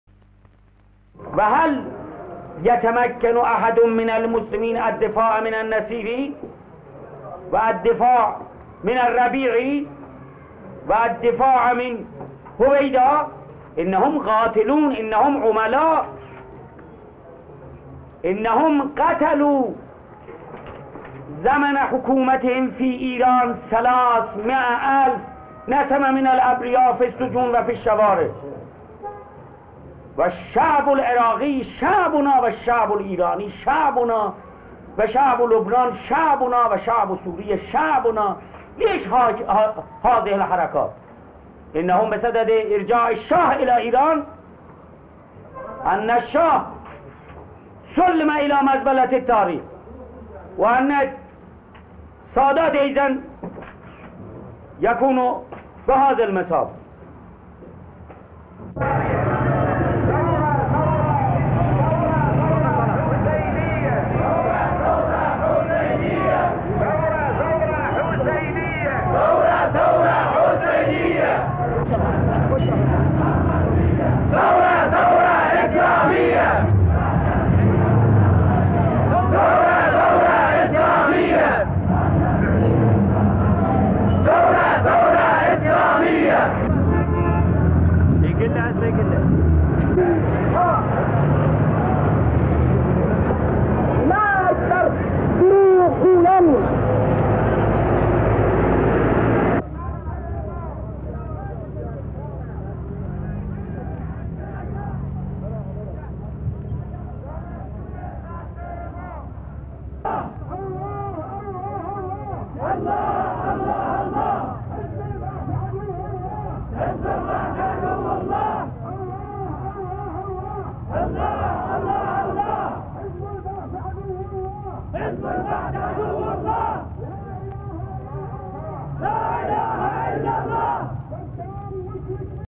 Ayatollah Khalkhali speaking in Arabic about the tension between Iran & Iraq, he was speaking during
(8 Apr 1980) 04/10/80   c0066987
 beirut & tehran.  Ayatollah Khalkhali speaking in Arabic about the tension between Iran & Iraq, he was speaking during his visit to Beirut, demo's including some Iraqis, marching in Tehran & chanting anti-Iraqi slogans.

 lnc19626 / 19630 "iran - iraq situ"

 (4/8/80 - 83ft - arabic sof & nat sof.)

 khalkhali, (ayatollah) (sof)
 anti - iraq - iran
 iran - fr - iraq
 iraq - fr - iran

 83 ft col pt